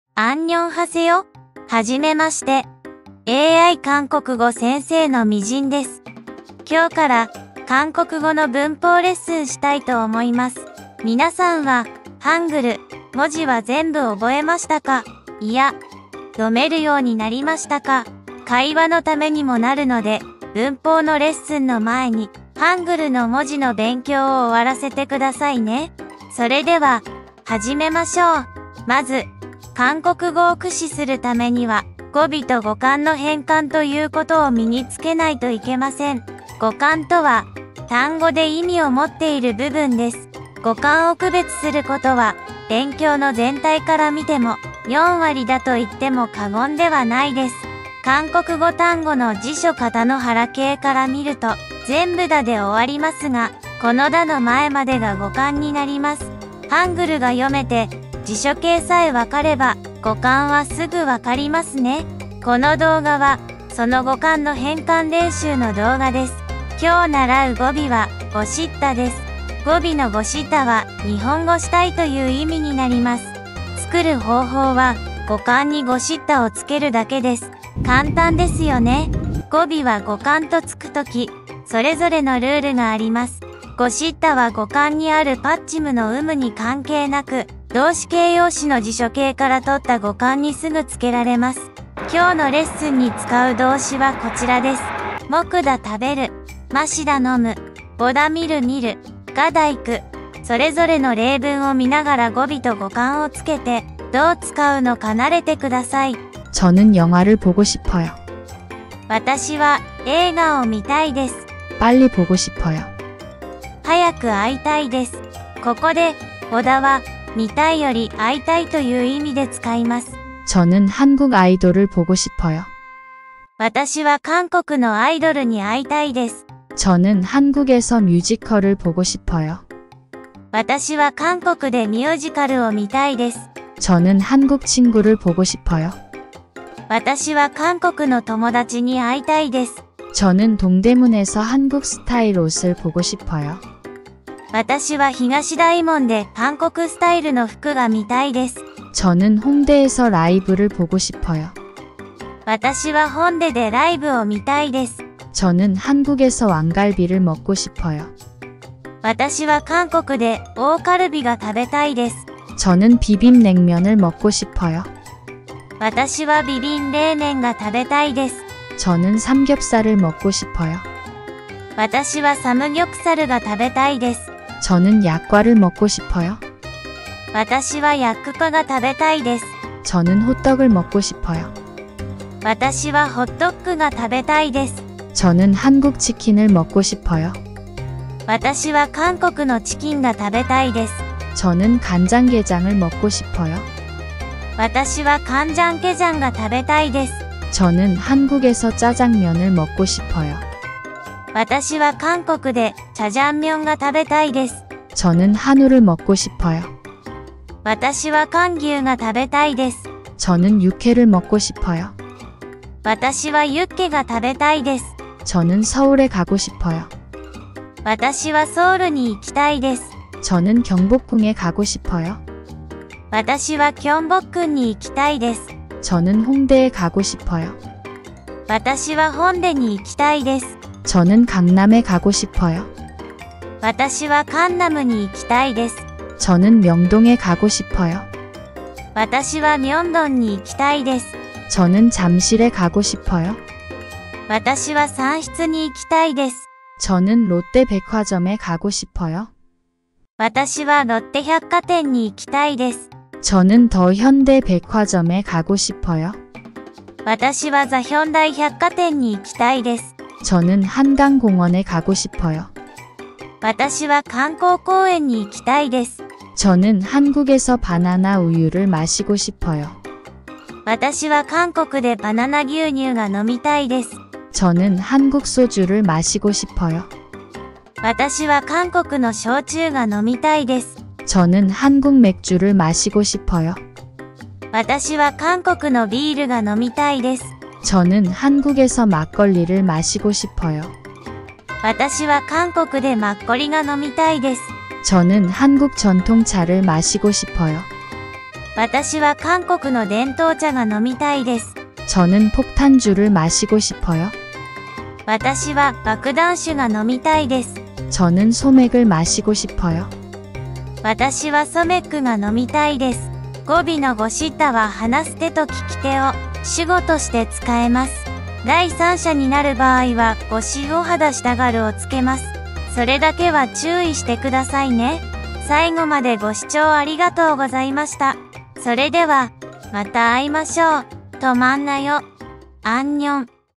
【韓国語の文法】lesson1.-したい「-고 싶다」with A.I.ミジン先生
lesson1. -したい「-고 싶다」
この動画は、その語幹の変換練習の動画です。
今日習う語尾は「고 싶다」です。語尾の「고 싶다」は日本語「-したい」という意味になります。「고 싶다」は語幹にあるパッチムの有無に関係なく、動詞・形容詞の辞書形からとった語幹にすぐつけられます。
今日のレッスンに使う動詞はこちらです
「먹다」　→　食べる
「마시다」　→　飲む
「보다」　→　観る・見る
「가다」　→　行く

★★動画で使った例文★★

저는 영화를 보고 싶어요
私は映画を観たいです。
빨리 보고 싶어요
早く会いたいです。
ここで、「보다」は、見たいより会いたいという意味で使います。
저는 한국 아이돌을 보고 싶어요
私は韓国のアイドルに会いたいです。
저는 한국에서 뮤지컬을 보고 싶어요
私は韓国でミュージカルを観たいです。
저는 한국 친구를 보고 싶어요
私は韓国の友達に会いたいです。
저는 동대문에서 한국 스타일 옷을 보고 싶어요
私は東大門で韓国スタイルの服が見たいです。
저는 홍대에서 라이브를 보고 싶어요
私はホンデでライブを観たいです。
저는 한국에서 왕갈비를 먹고 싶어요
私は韓国で王カルビが食べたいです。
저는 비빔 냉면을 먹고 싶어요
私はビビン冷麺が食べたいです。
저는 삼겹살을 먹고 싶어요
私はサムギョプサルが食べたいです。
저는 약과를 먹고 싶어요
私はヤッククァが食べたいです。
저는 호떡을 먹고 싶어요
私はホットックが食べたいです。
저는 한국 치킨을 먹고 싶어요
私は韓国のチキンが食べたいです。
저는 간장게장을 먹고 싶어요
私はカンジャンケジャンが食べたいです。
저는 한국에서 짜장면을 먹고 싶어요
私は韓国でチャジャンミョンが食べたいです。
저는 한우를 먹고 싶어요
私は韓牛が食べたいです。
저는 육회를 먹고 싶어요
私はユッケが食べたいです。
저는 서울에 가고 싶어요
私はソウルに行きたいです。
저는 경복궁에 가고 싶어요
私は景福宮に行きたいです。
저는 홍대에 가고 싶어요
私はホンデに行きたいです。
저는 강남에 가고 싶어요
私はカンナムに行きたいです。
저는 명동에 가고 싶어요
私は明洞に行きたいです。
저는 잠실에 가고 싶어요
私は蚕室に行きたいです。
저는 롯데 백화점에 가고 싶어요
私はロッテ百貨店に行きたいです。
저는 더 현대 백화점에 가고 싶어요
私はザヒョンダイ百貨店に行きたいです。
저는 한강 공원에 가고 싶어요
私は漢江公園に行きたいです。
저는 한국에서 바나나 우유를 마시고 싶어요
私は韓国でバナナ牛乳が飲みたいです。
저는 한국 소주를 마시고 싶어요
私は韓国の焼酎が飲みたいです。
저는 한국 맥주를 마시고 싶어요
私は韓国のビールが飲みたいです。
저는 한국에서 막걸리를 마시고 싶어요
私は韓国でマッコリが飲みたいです。
저는 한국 전통 차를 마시고 싶어요
私は韓国の伝統茶が飲みたいです。
저는 폭탄주를 마시고 싶어요
私は爆弾酒が飲みたいです。
저는 소맥을 마시고 싶어요
私はソメックが飲みたいです。